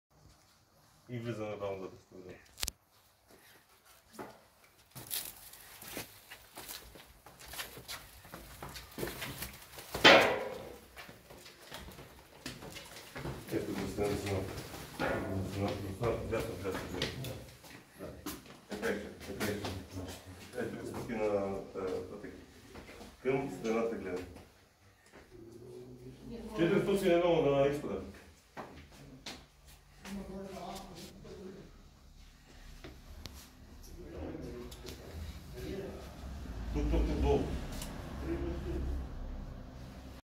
Задържан мъж при спецакция "купен вот" в Сунгурларе
Акцията е част от мащабната национална полицейска операция срещу престъпленията свързани с политическите права на гражданите.

Автор на кадрите: МВР Бургас